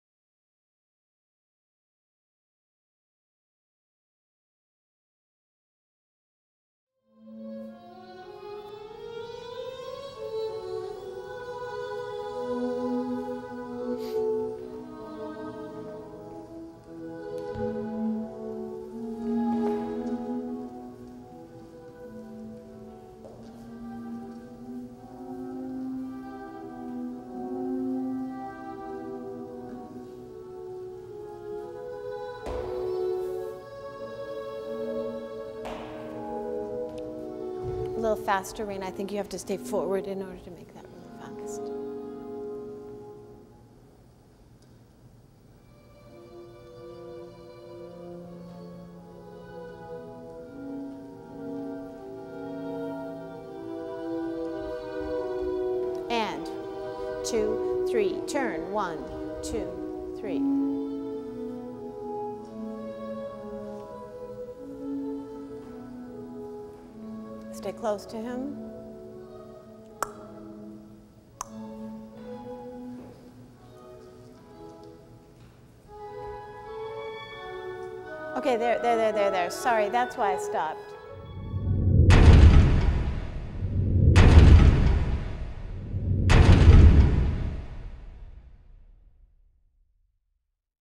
Point of Departure Preview
Lise Houlton of Minnesota Dance Theatre directs  a company rehearsal for the upcoming production of "Point of Departure".